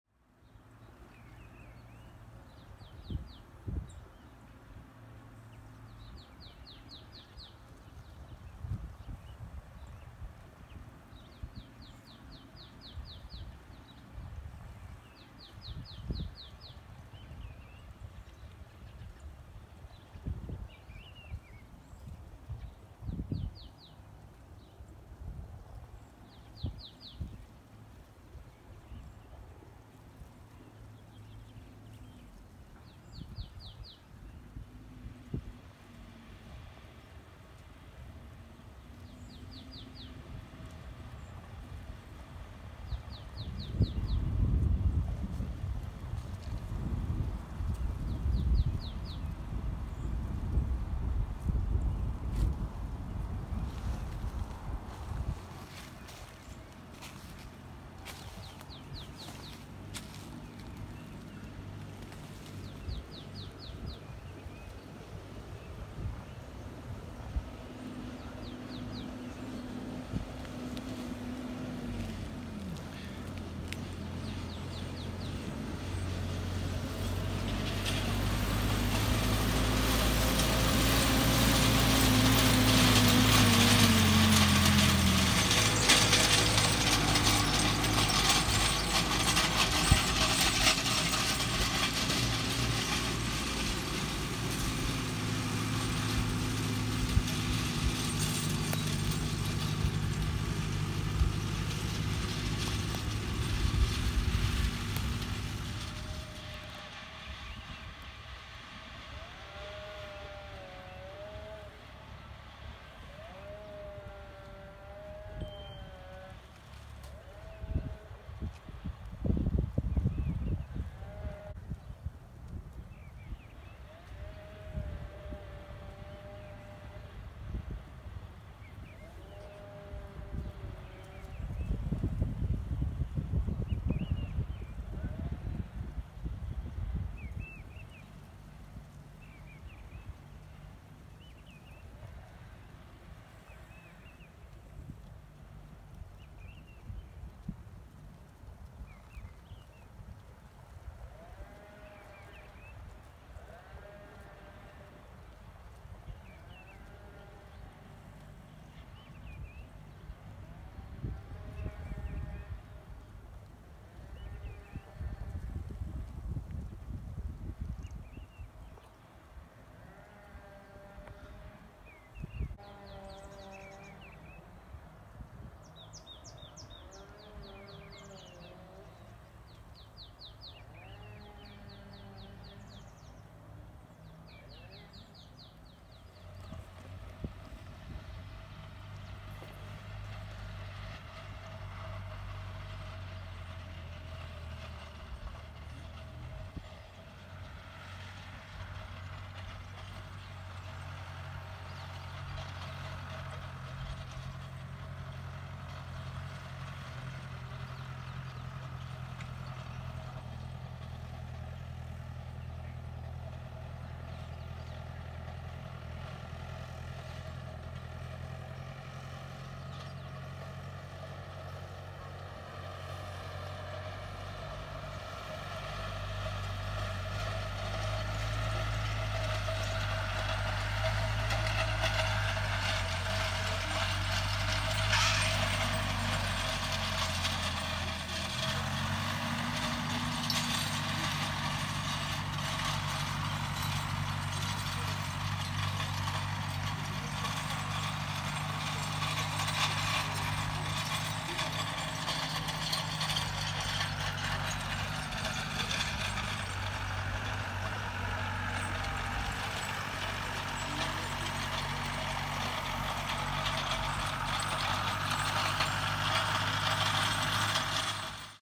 Wiesen abschleppen Grassland harrowing
Hier sieht man den BCS Hangtraktor Volcan 950 beim Abschleppen der steilen Wiesen.
Here you can see the BCS slope tractor Volcan 950 harrowing the steep meadows.